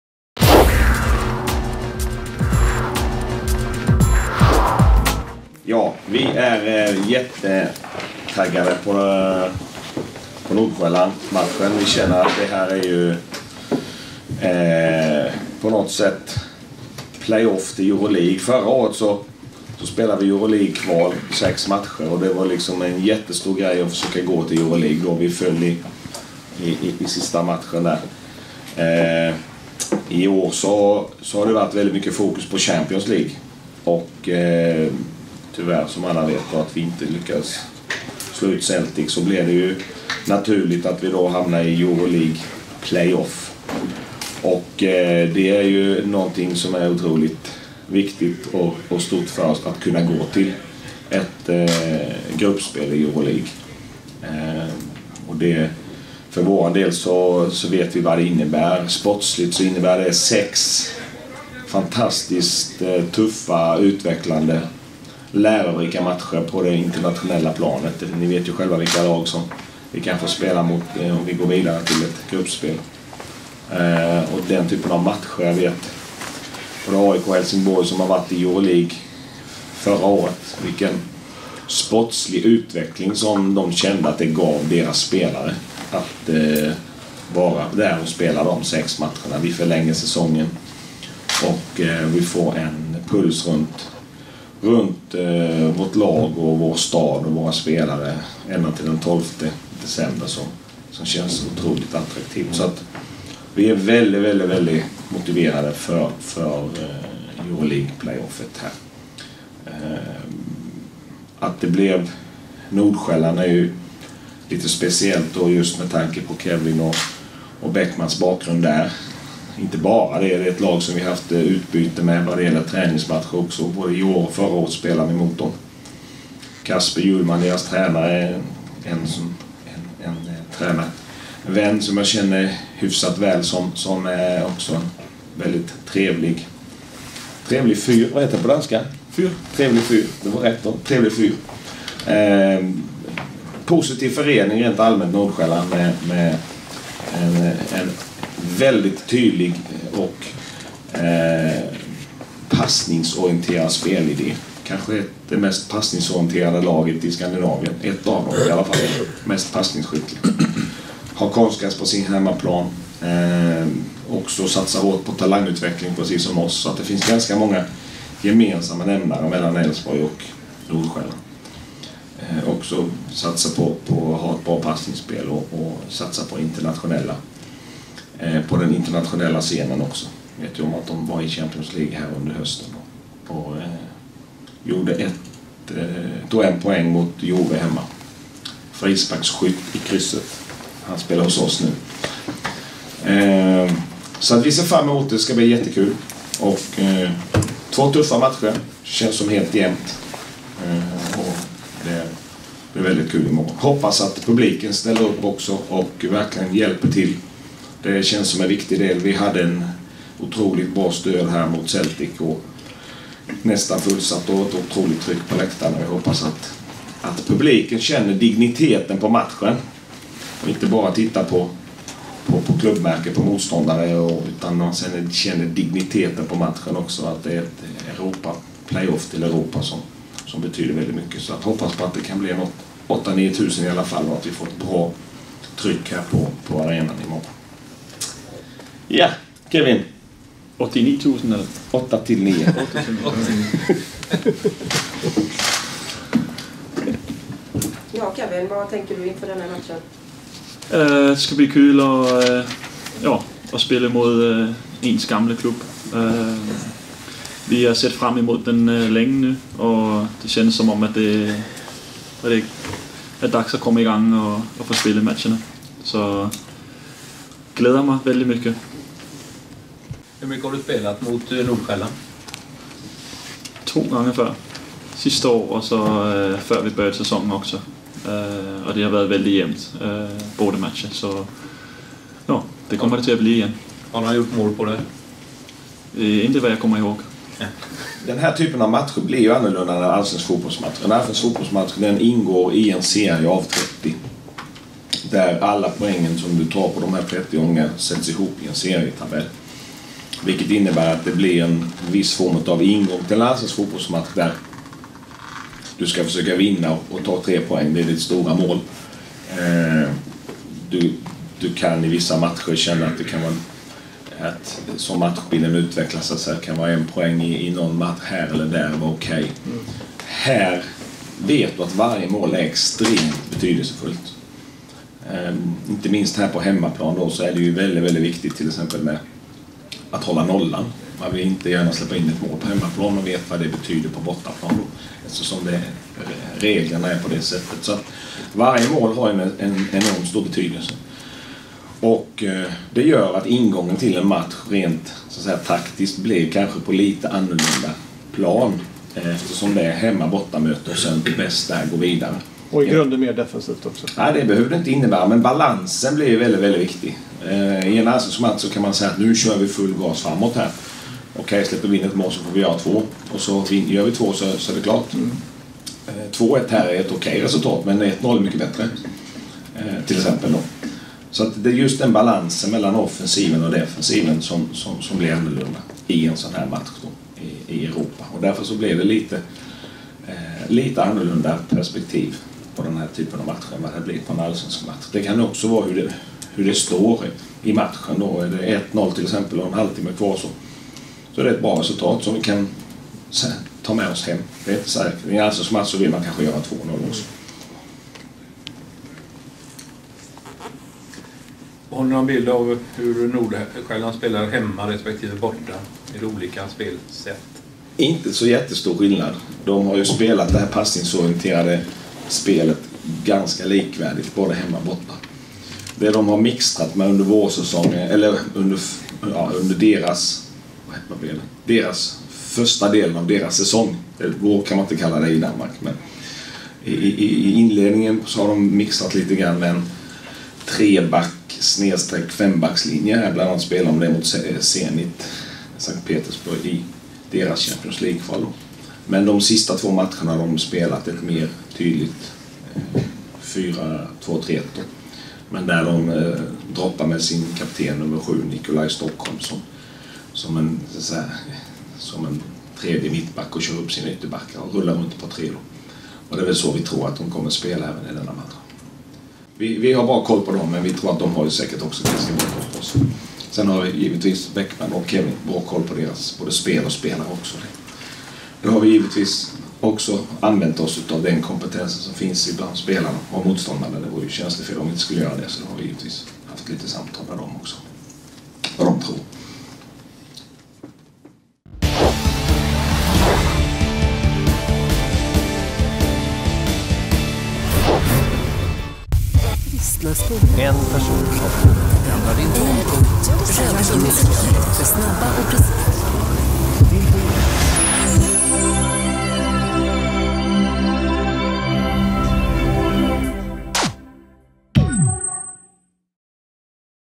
Elfsborg inför FC Nordsjälland i Europa League.
En dag kvar innan Elfsborg tar sig an FC Nordsjälland i den första matchen i Europa League. Se och hör Jörgen Lennartsson och Kevin Stuhr - Ellegaard från dagens presskonferens på Borås Arena.